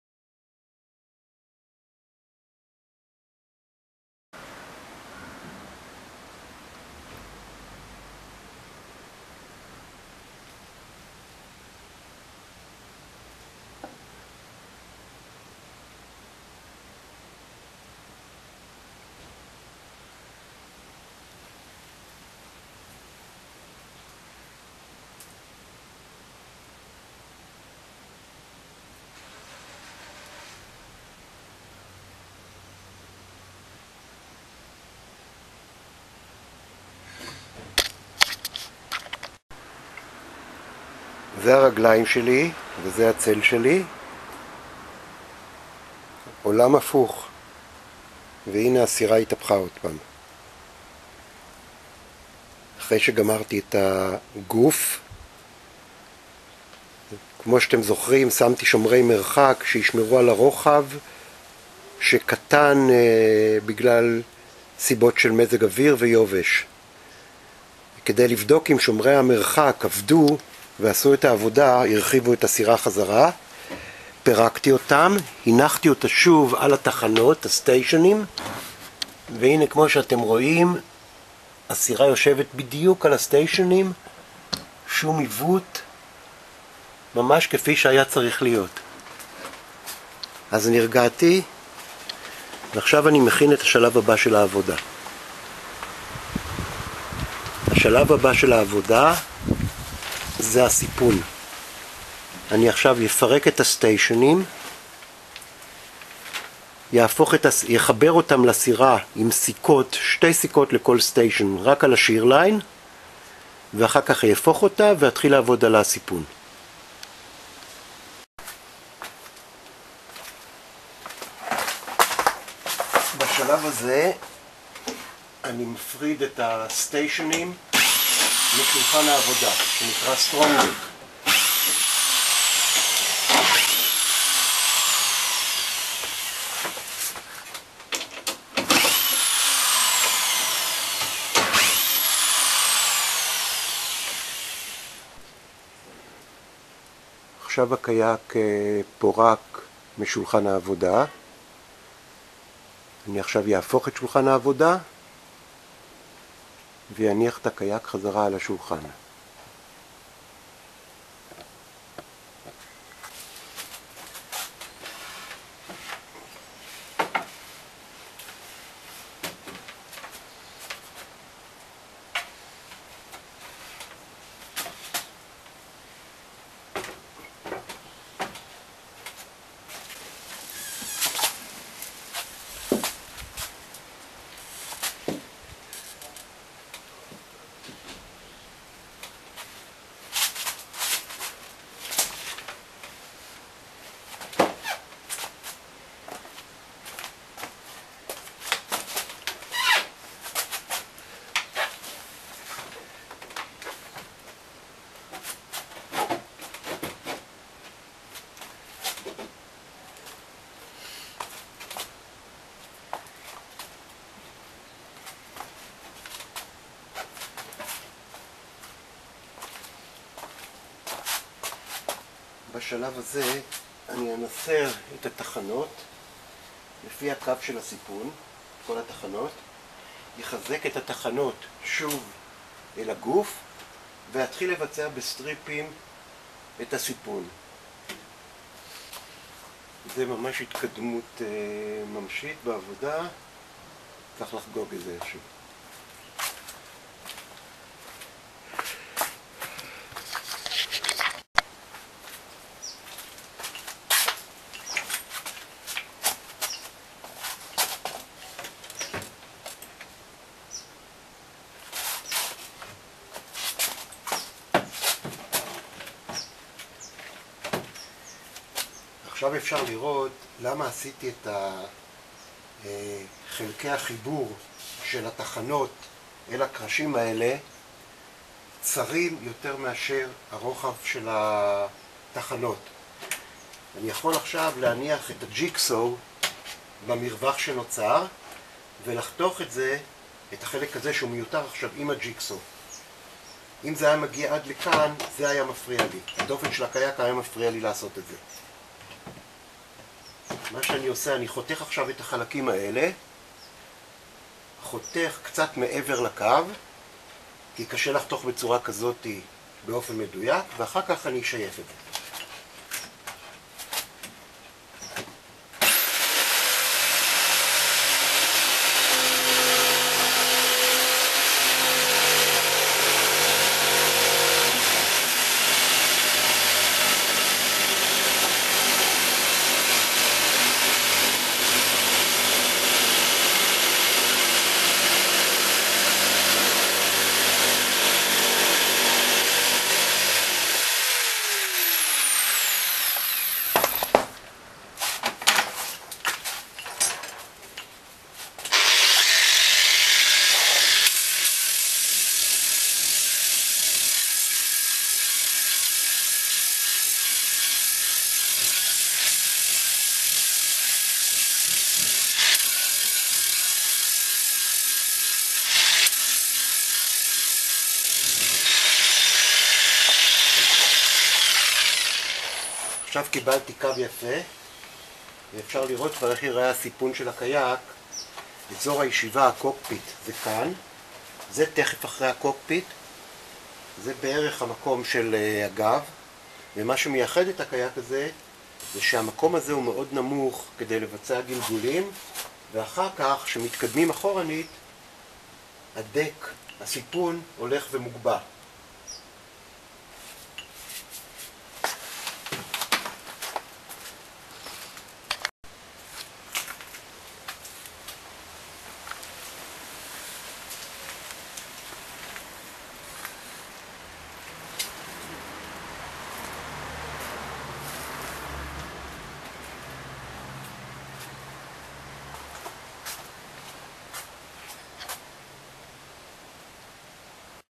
Preparations for the deck striping.wmv
It will take like half a day to change the state of mind for the deck stripping. Decisions, options, checking the stock of wood etc. But once I shall finish the sheerline strips, it will run like a sprinter.